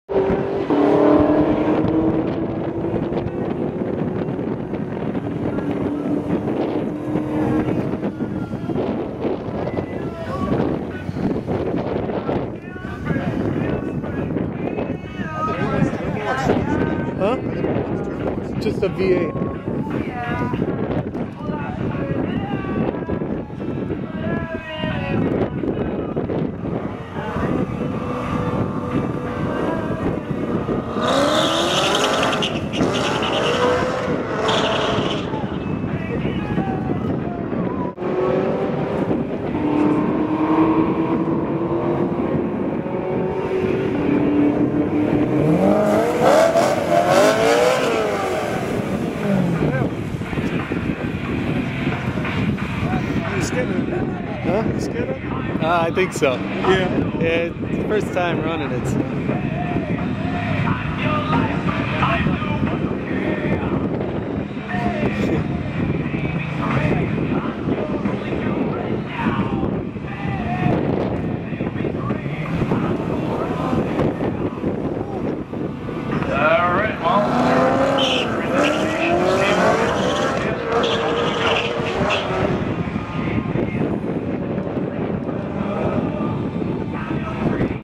Unedited Video of Rickys 2008 E92 M3 at GLD
An unedited video of two 1/4 mile passes made at GLD that were both aborted due to wheel hop in the 2008 BMW M3 E92.